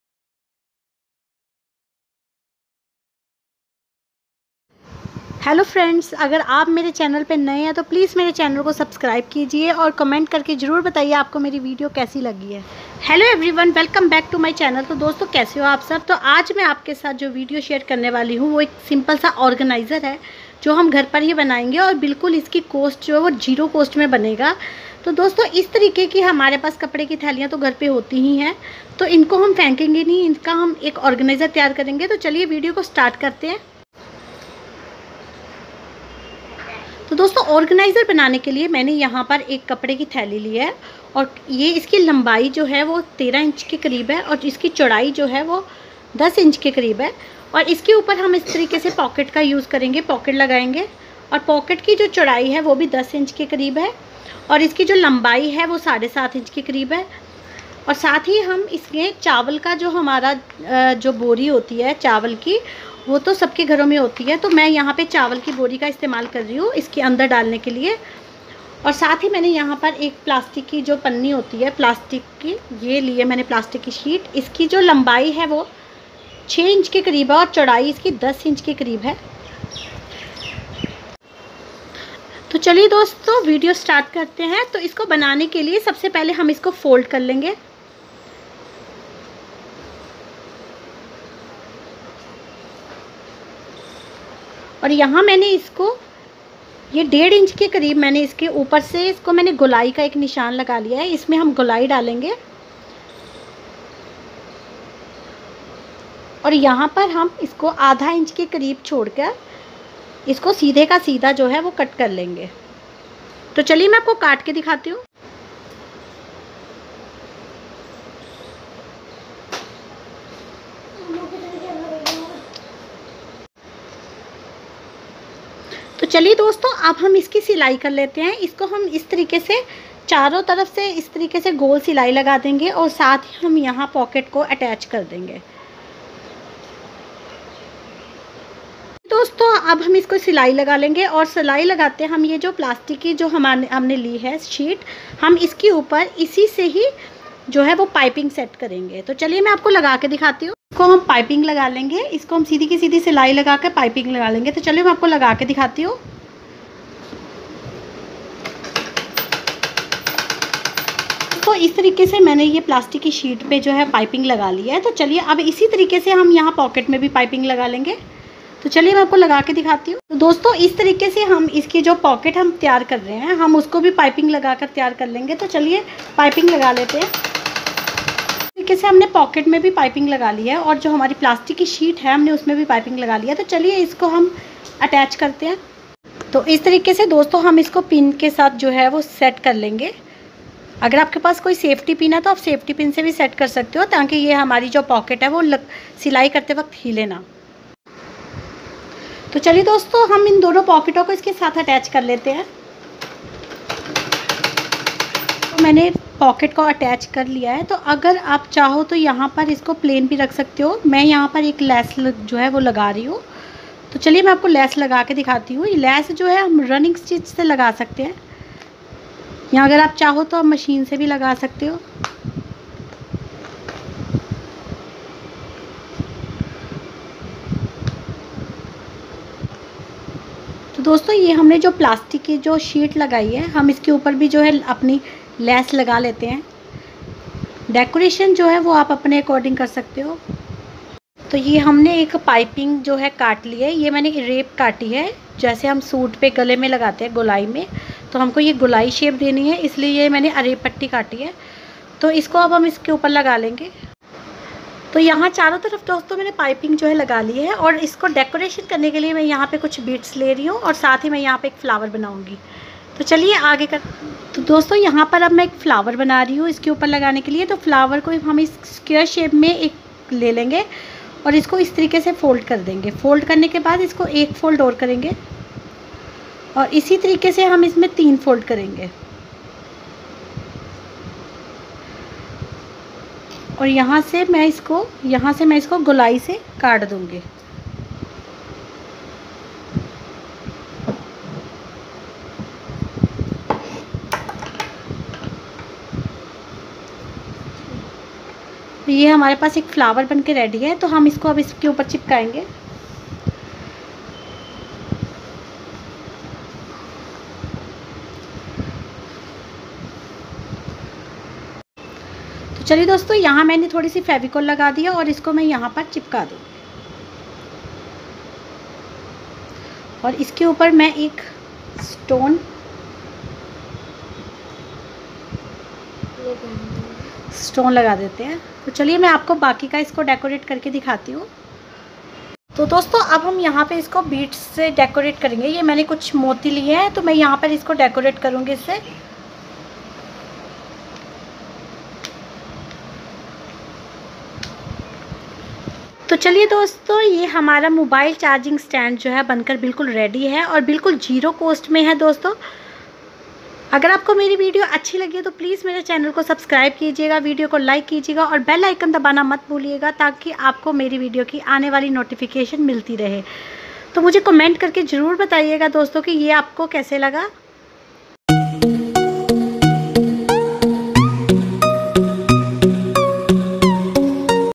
Diy making a mobile charging holder👍
mobile charging holder
preparation time- 20 min
very easy  to make and useful
0 cost